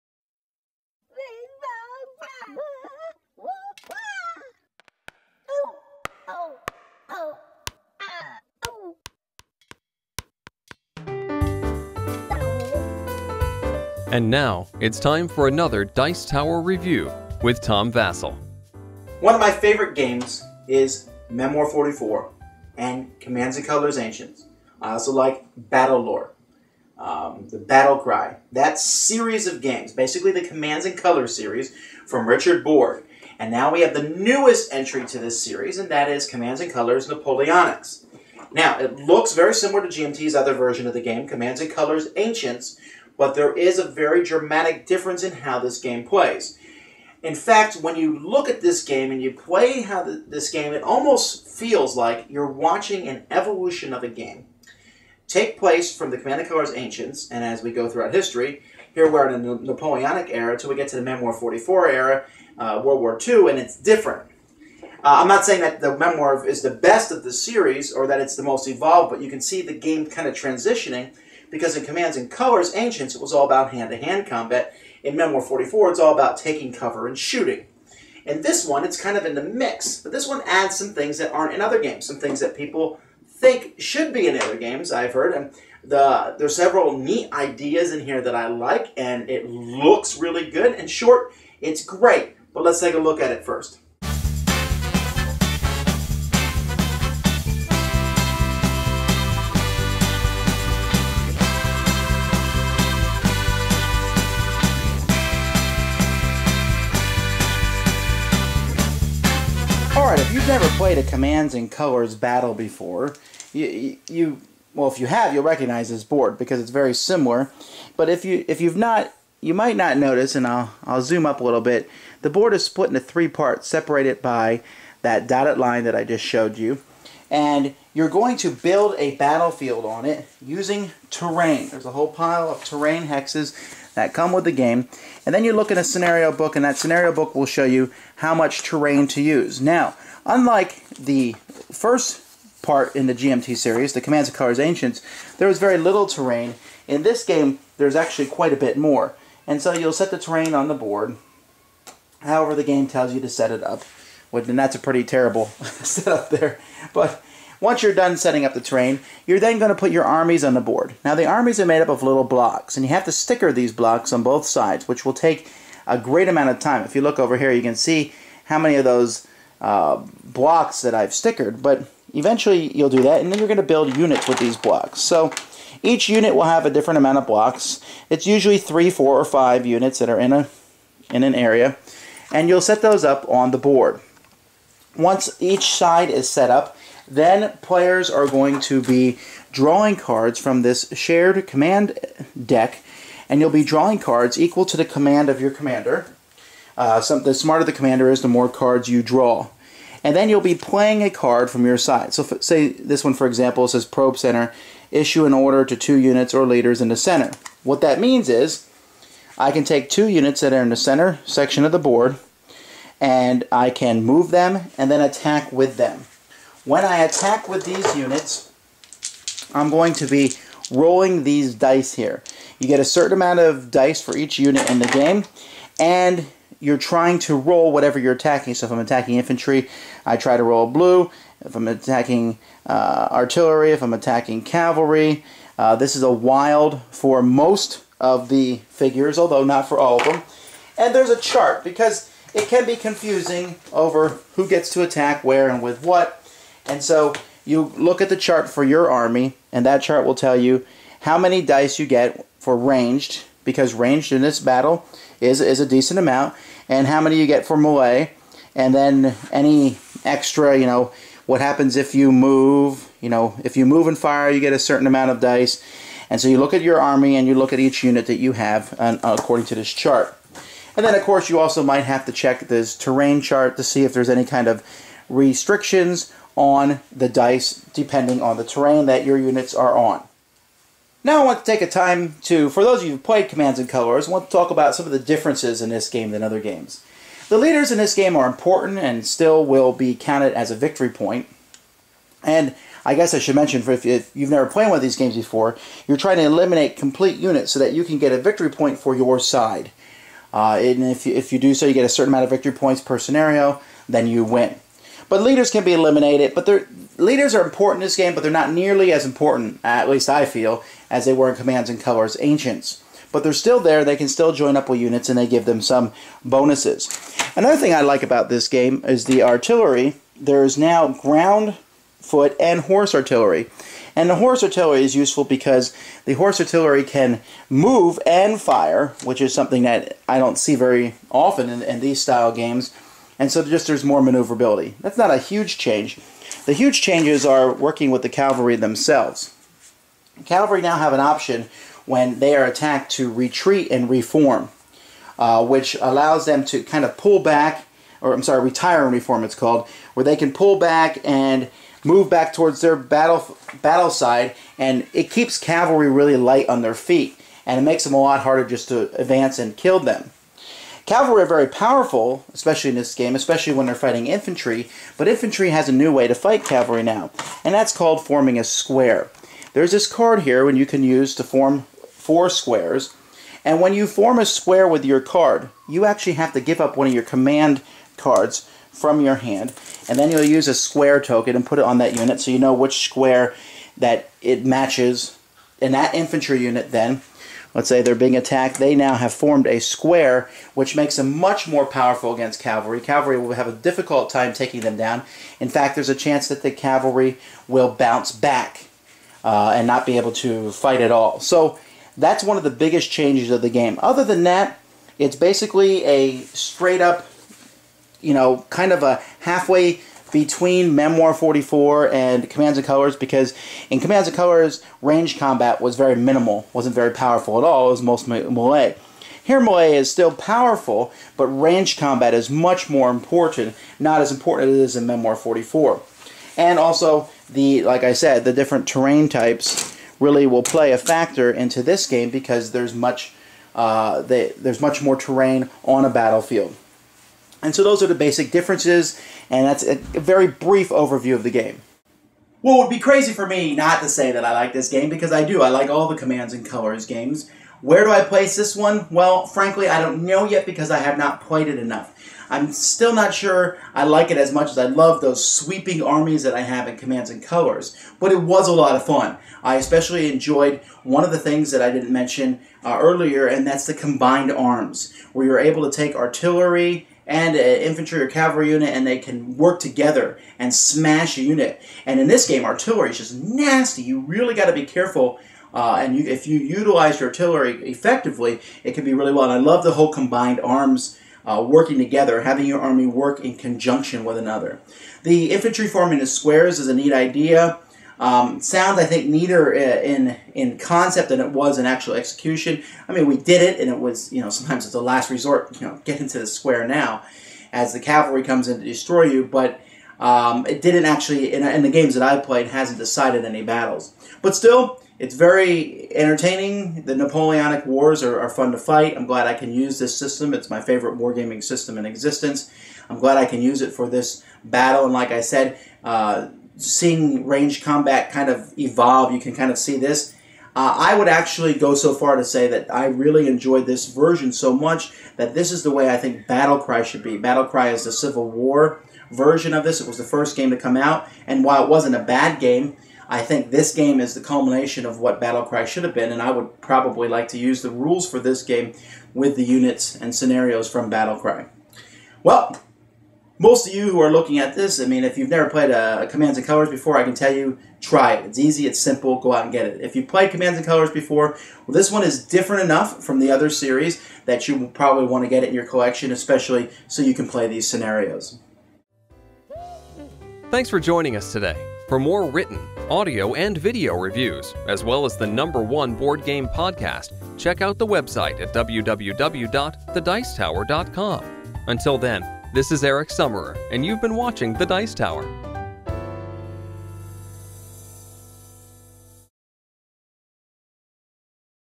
Commands and Colors Napoleonic Wars Review - with Tom Vasel
Tom Vasel reviews Commands and Colors: Napoleonic Wars from GMT games

00:00 - Introduction
01:50 - Game overview
07:21 - Final thoughts